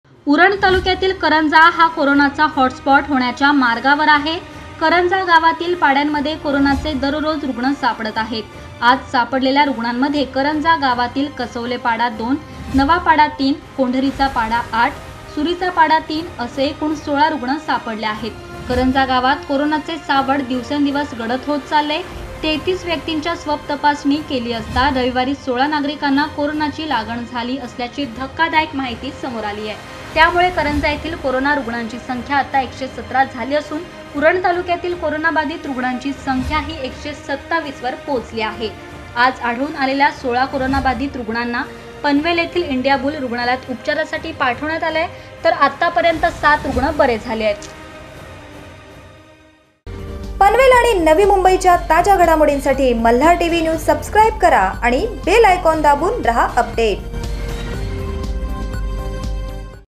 उरण तालुक्यातील कंरजा गाव कोरोनाचा हॉटस्पॉट होण्याच्या मार्गावर !
malhartvnews at panvel | उरण तालुक्यातील कंरजा गाव कोरोनाचा हॉटस्पॉट होण्याच्या मार्गावर;कंरजा गावातील कसवले पाडा २ ,नवापाडा ३ ,कोंढरीचा पाडा ८ ,सुकरीचा पाडा ३ असे एकूण १६ नवे रुग्ण;कंरजा गावातील कोरोना बाधितांची संख्या गेली  ११७ वर !